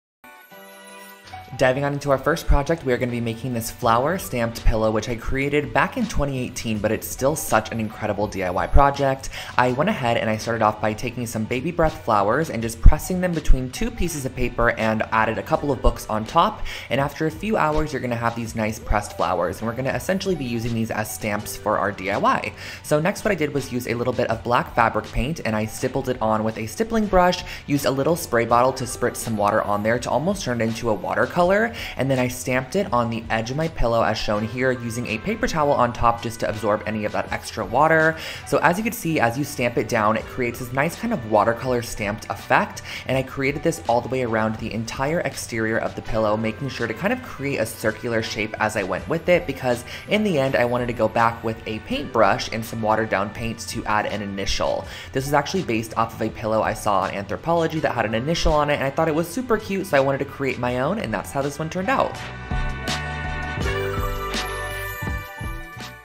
How to make a flower stamped pillow | Room décor | Easy DIY projects ideas | 1/100
A very easy and nice DIY project, to make a flower stamped pillow for room decorations.

ATTRIBUTION:

Original Title:
100 DIY HOME DECOR IDEAS + HACKS You Actually Want To Make! ✨ (Full Tutorials)